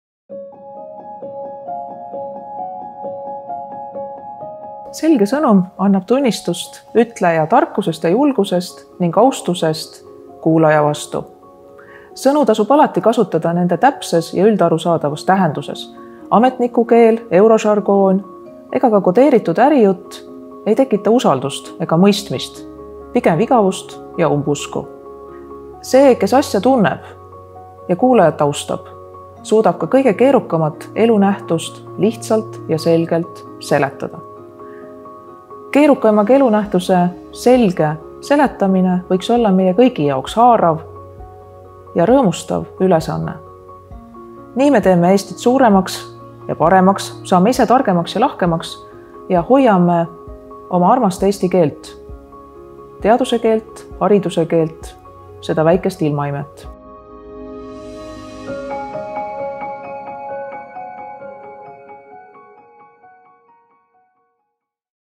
Selge sõnum 2022. Õiguskantsler Ülle Madise
Võistluse ja selge keele mõtteviisi patrooni õiguskantsler Ülle Madise sõnul annab selge sõnum tunnistust ütleja tarkusest ja julgusest ning austusest kuulaja vastu: „Ametnikukeel, eurožargoon ega kodeeritud jutt ei tekita mõistmist, pigem igavust ja umbusku. Olgu keerukamagi info edastamine meie kõigi jaoks rõõmustav ja haarav ülesanne.“

Kutsume üles märkama ja tunnustama neid ettevõtteid ja organisatsioone, kes väärtustavad ilusat eesti keelt ning kelle ühiskondlikud sõnumid on selged ja arusaadavad.